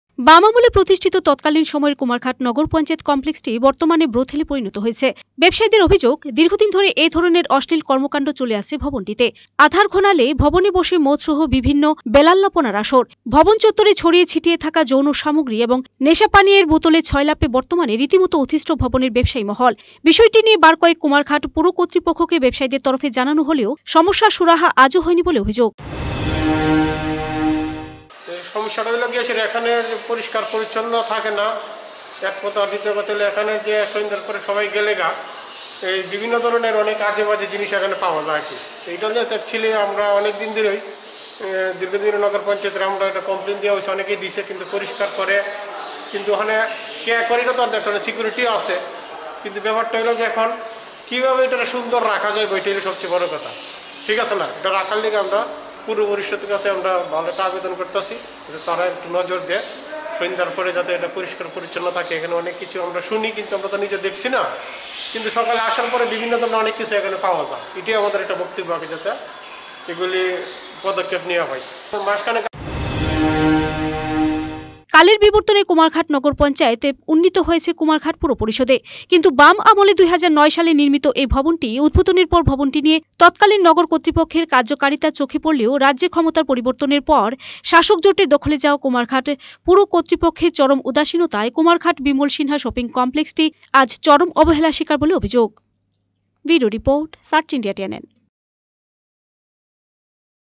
অঘোষিত ব্রথেল#
বাম আমলে প্রতিষ্ঠিত তৎকালীন সময়ের কুমারঘাট নগর পঞ্চায়েত কমপ্লেক্সটি বর্তমানে ব্রথেলে পরিনত হয়েছে ।ভবনটি দেখভালের অভাবে একেতো পরিনত হয়েছে ভুতুড়ে বাড়ীতে তারমাঝে রয়েছে নিশিকুটুম্বদের দৌরাত্ব ।